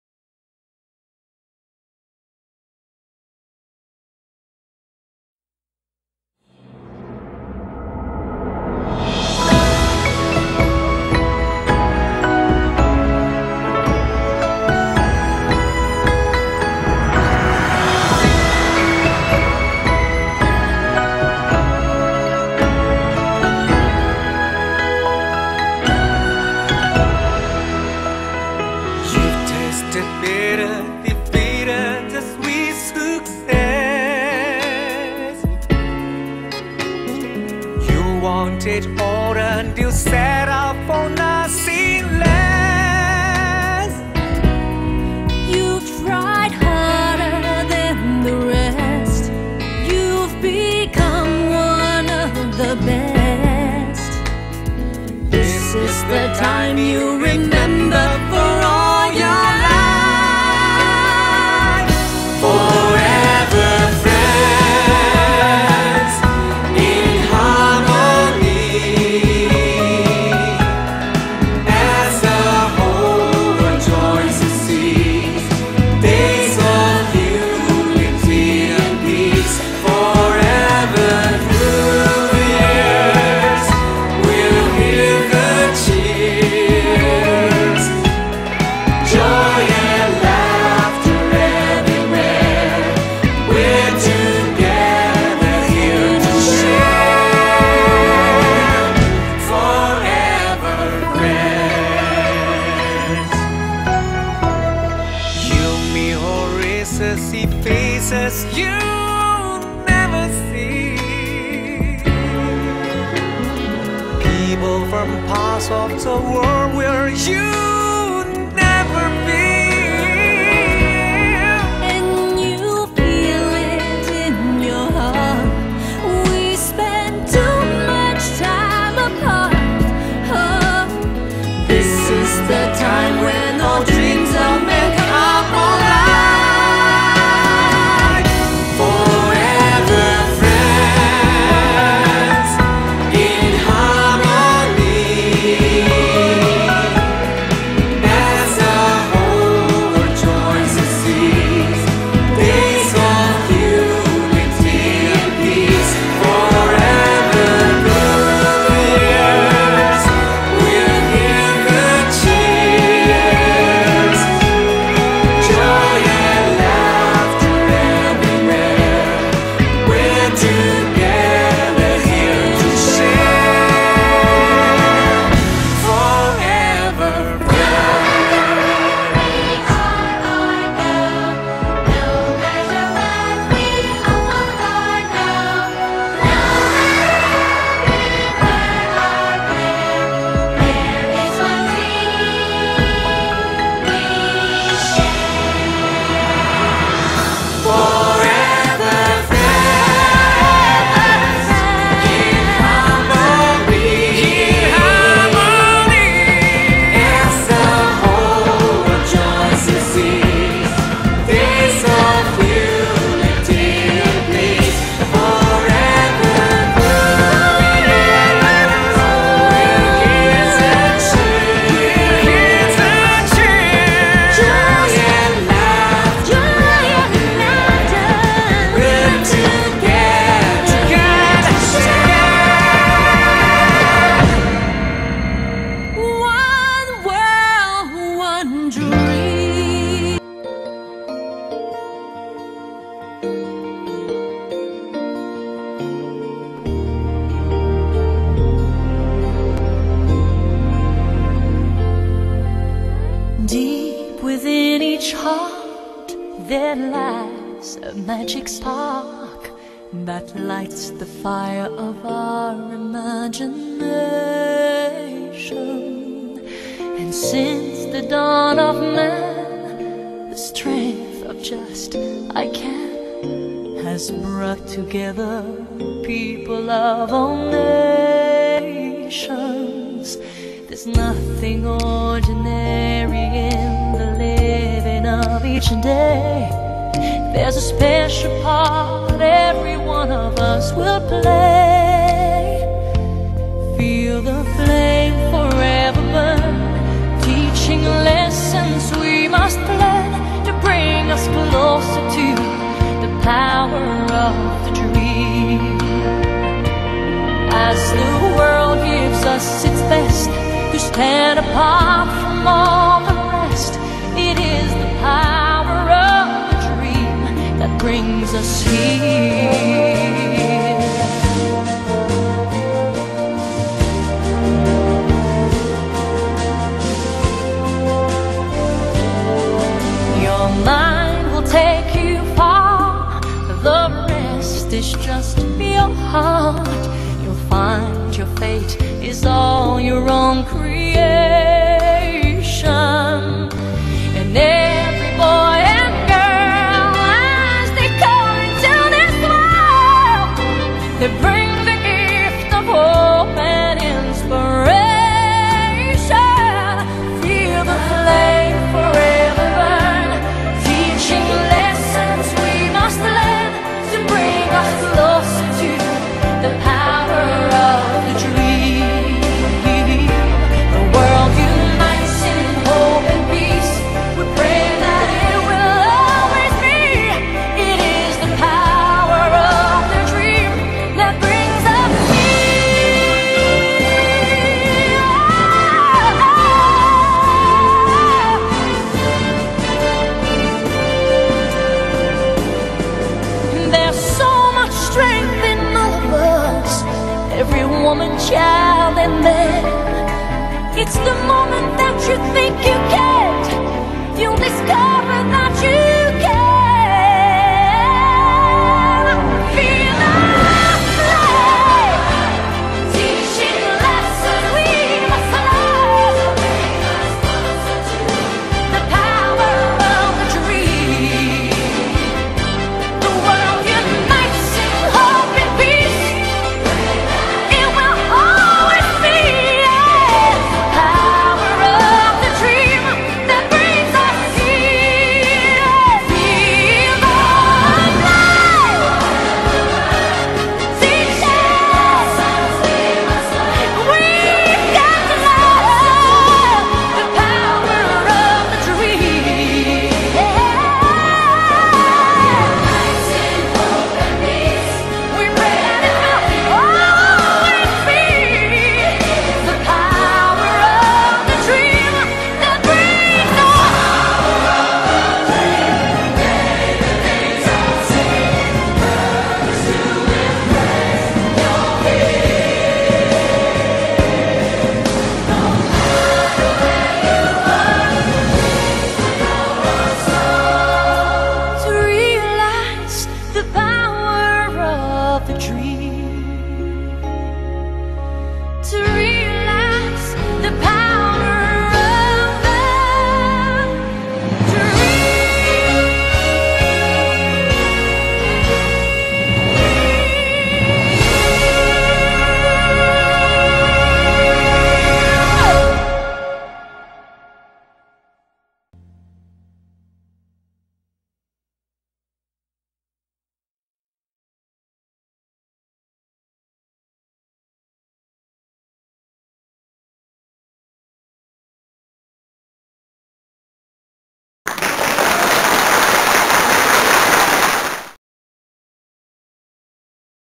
A Tribute To Beijing Olympic Games 2008 Ver.1
A FREE video clip dedicated to all athletes of all countries who participated in the Beijing Olympic Games 2008,their family members,friends,government leaders and Everyone who share the One World One Dream to strive for a better future for Mankind.